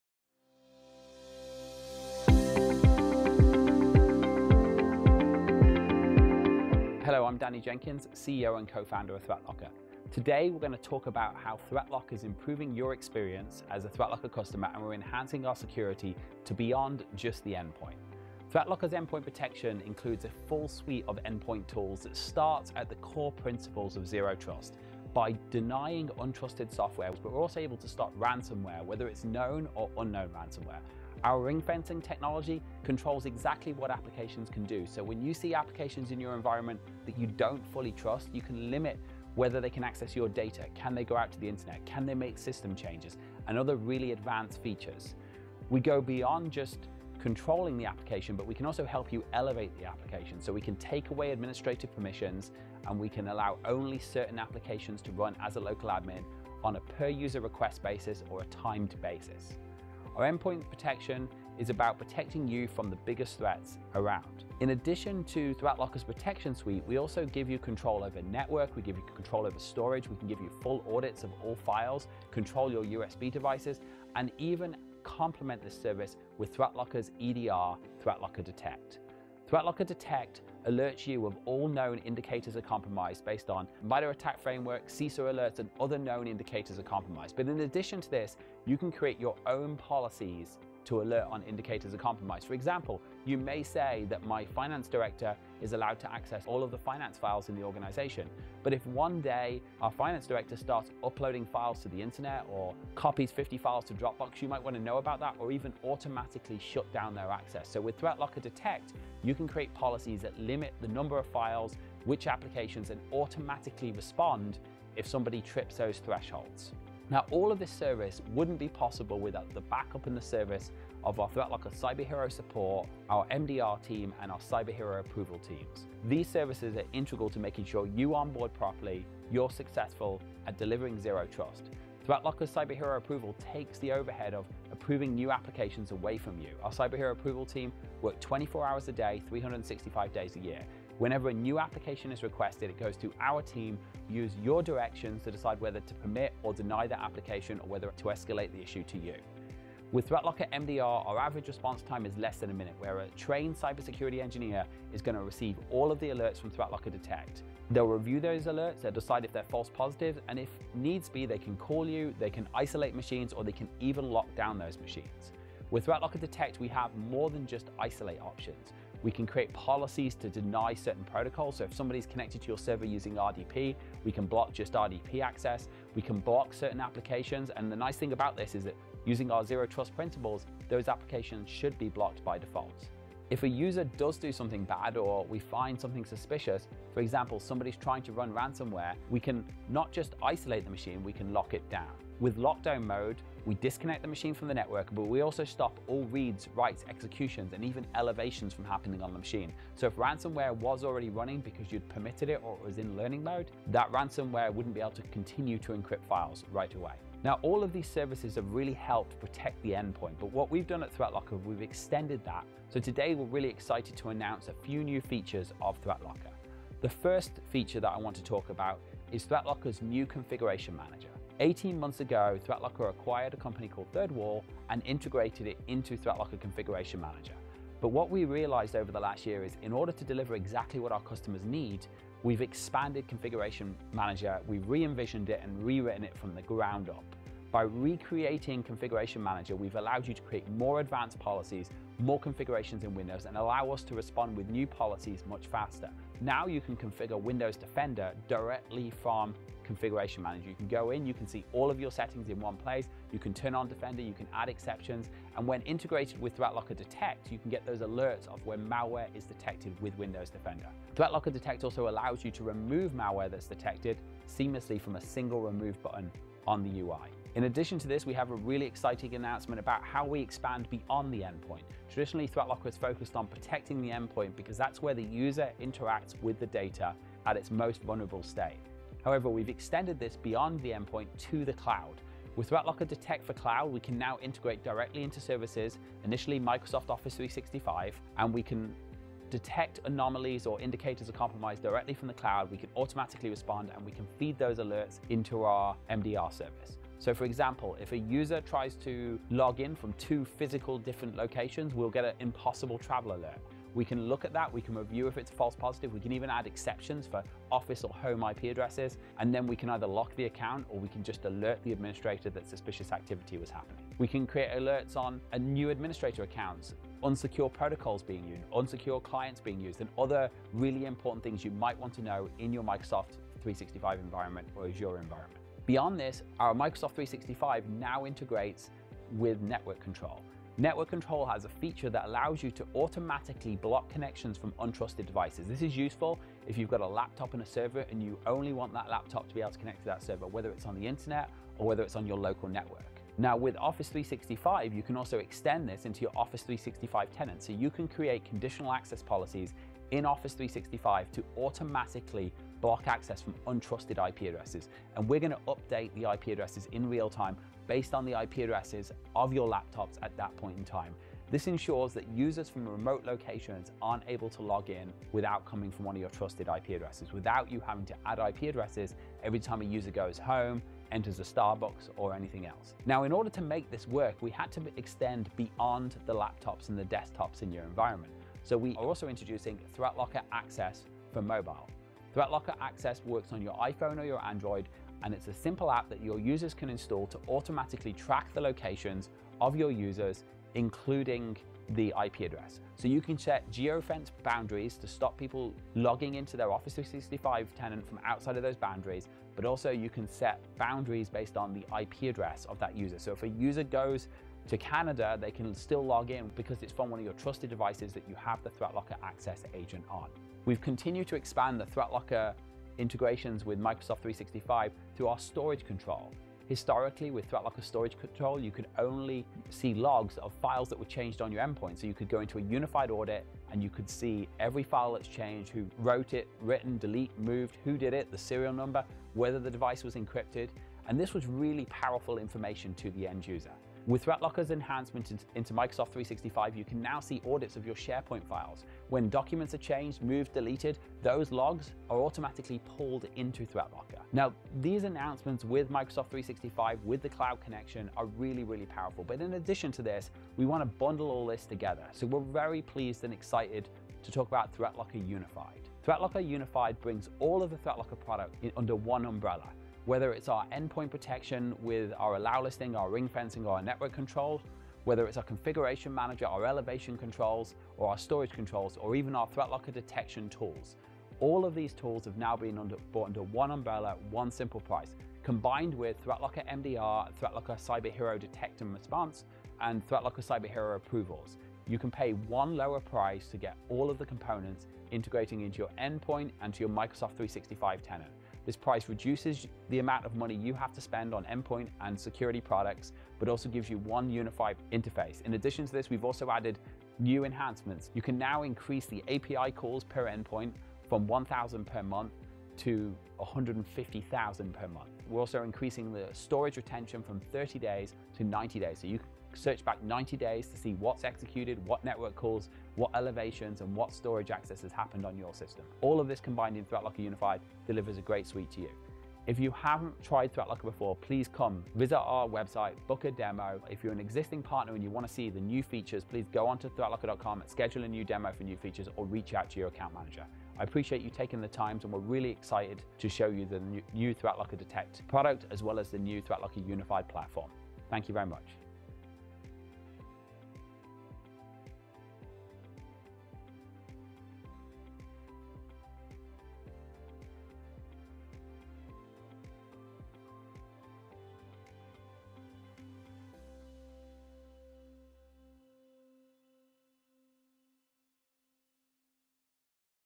ThreatLocker® MAJOR Announcement - May 2024
Danny Jenkins announces MAJOR changes coming to the ThreatLocker® Zero Trust Endpoint Protection Platform.

About:
ThreatLocker® is a Zero Trust endpoint protection platform that provides enterprise-level cybersecurity to organizations globally. With ThreatLocker®, you can allow only what you need and block everything else, including ransomware!

00:00 - Introduction
00:18 - What does ThreatLocker® do?
01:57 - ThreatLocker® Cyber Hero® Support, MDR, and Approvals
03:35 - (NEW) Configuration Manager and Windows Defender
04:15 - (NEW) ThreatLocker® Detect and Windows Defender
04:27 - (NEW) ThreatLocker® expanding to the Cloud
05:26 - (NEW) Network Control and Microsoft 365
06:12 - (NEW) ThreatLocker® Access for Mobile
07:24 - (NEW) ThreatLocker® Unified
08:39 - How to learn more about the update to ThreatLocker®
08:59 - Outro